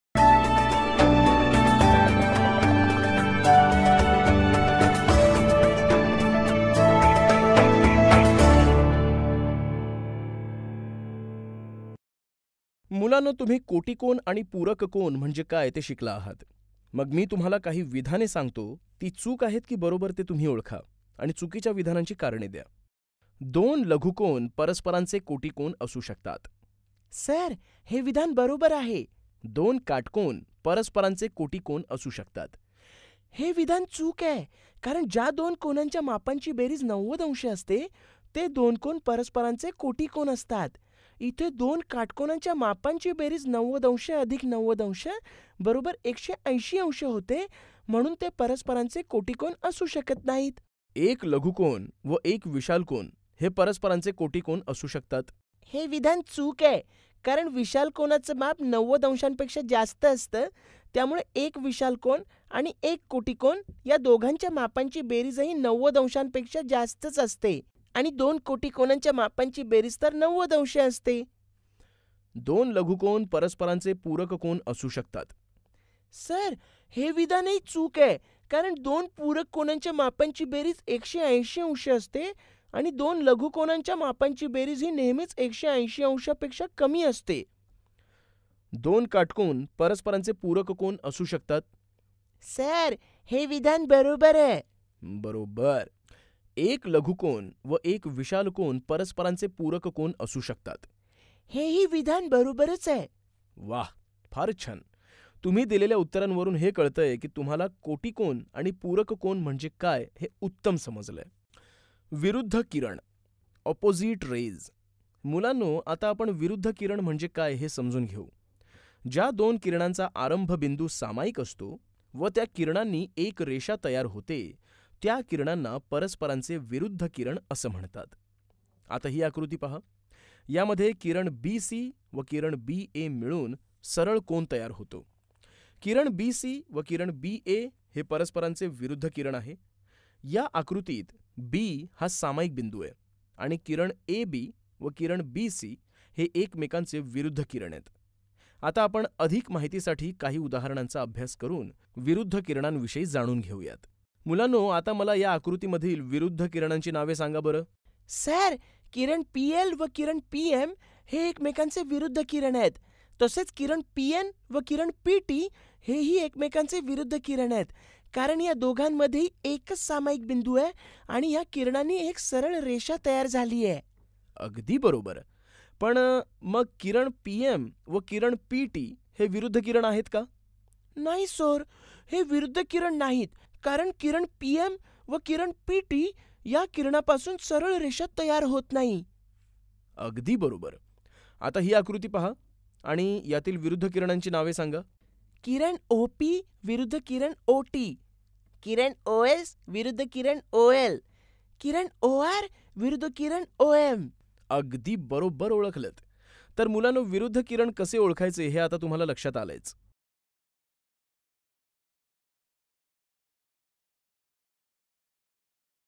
7th Maths | Chapter#04 | Topic#05 | चुकीच्या विधानांची कारणे द्या. | Marathi Medium
मुलांनो तुम्ही कोटिकोन आणि पूरक कोन म्हणजे काय ते शिकला आहात. मग मी तुम्हाला काही विधाने सांगते. ती चूक आहेत की बरोबर ते तुम्ही ओळखा. आणि चुकीच्या विधानांची कारणे द्या. शि: दोन लघुकोन परस्परांचे कोटिकोन असू शकतात = वि: सर हे विधान बरोबर आहे. शि: दोन काटकोन परस्परांचे कोटिकोन असू शकतात = वि: हे विधान चूक आहे. कारण ज्या दोन कोनांच्या मापांची बेरीज 90 अंश असते, ते दोन कोन परस्परांचे कोटिकोन असतात. येथे दोन काटकोनांच्या मापांची बेरीज 90अंश + 90अंश =180अंश होते. म्हणून ते परस्परांचे कोटिकोन असू शकत नाही. शि: एक लघुकोन व एक विशालकोन हे परस्परांचे कोटिकोन असू शकतात. वि: हे विधान चूक आहे. कारण विशाल कोनाचे माप 90अंश पेक्षा जास्त असते. त्यामुळे एक विशाल कोन आणि एक कोटिकोन या दोघांच्या मापांची बेरीजही 90अंश पेक्षा जास्तच असेल. आणि दोन कोटिकोनांच्या मापांची बेरीज तर 90० असते.